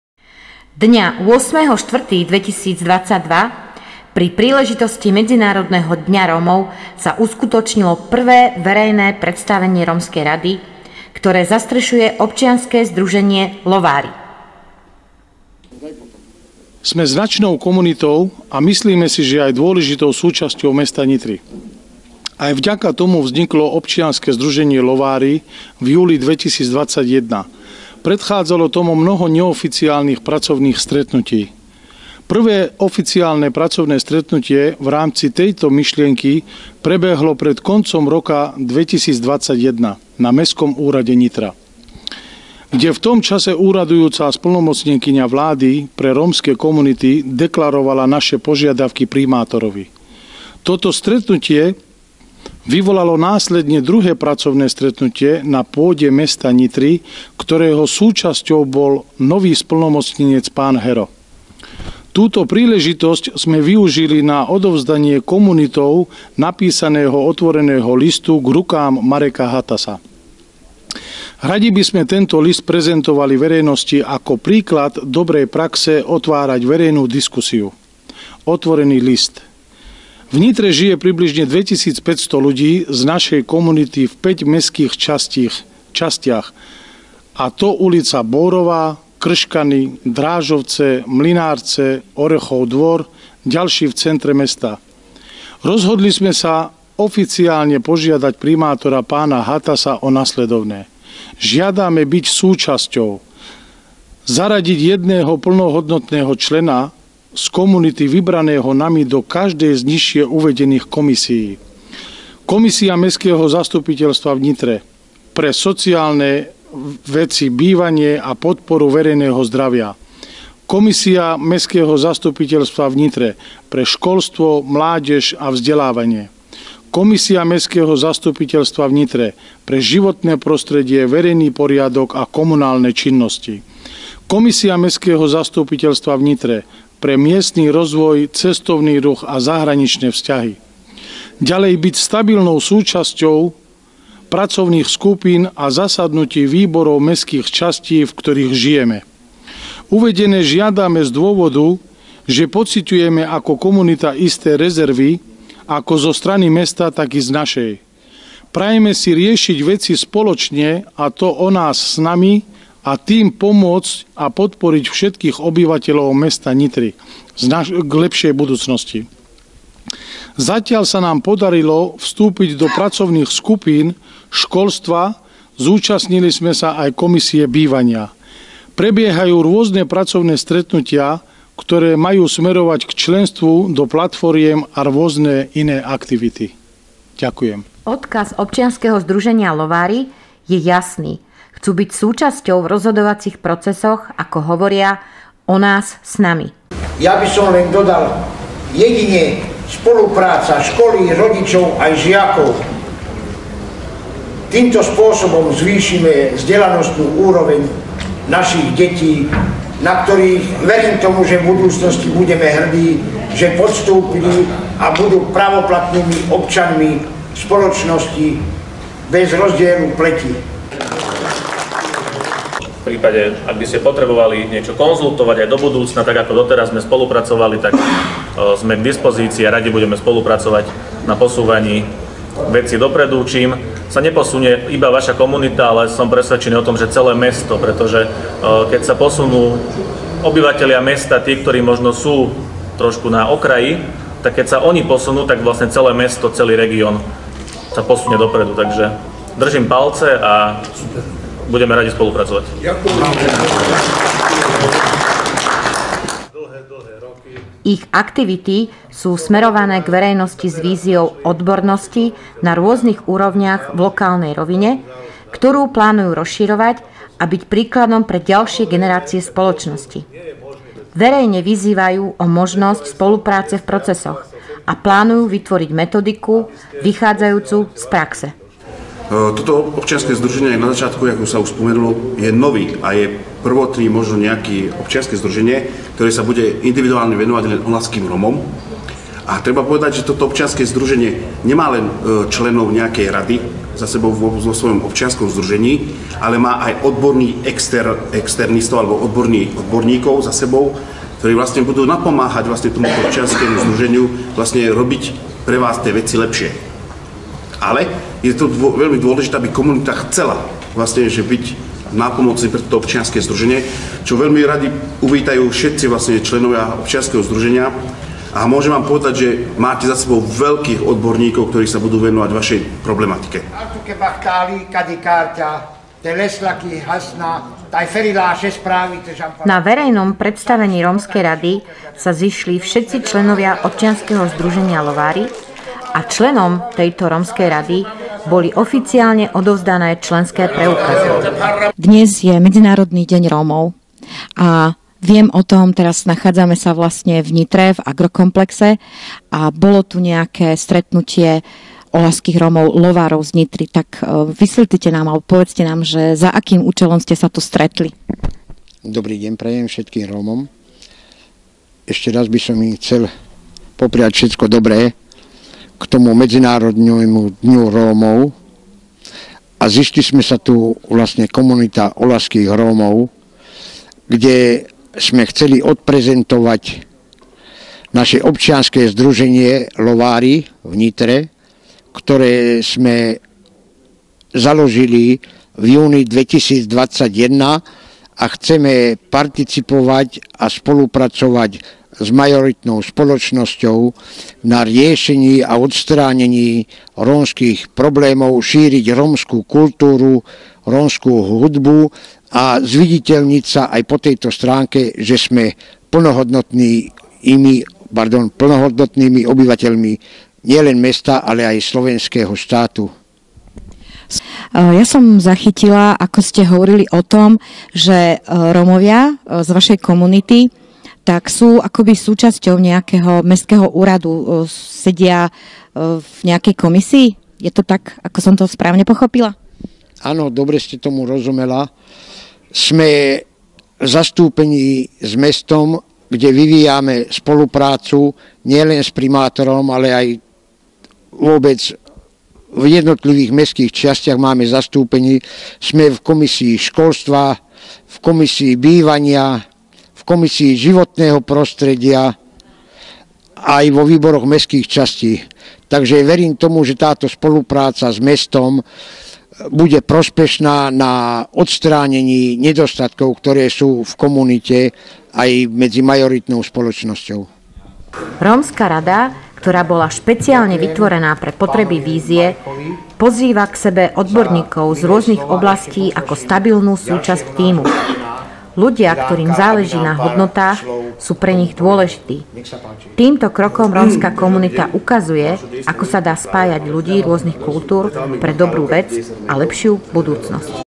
Tv Romana - Verejné predstavenie rómskej rady OZ LOVÁRI
OZ LOVÁRI - jeho členovia chcú byť súčasťou v rozhodovacích  procesoch ako hovoria,, O nás s nami,,  Ich aktivity sú smerované k verejnosti s víziou odbornosti na rôznych úrovniach v lokálnej rovine, ktorú plánujú rozširovať a byť príkladom pre ďalšie generácie spoločnosti. Verejné vyzývajú na možnosť spolupráce v  procesoch a plánujú vytvoriť metodiku vychádzajúcu z praxe.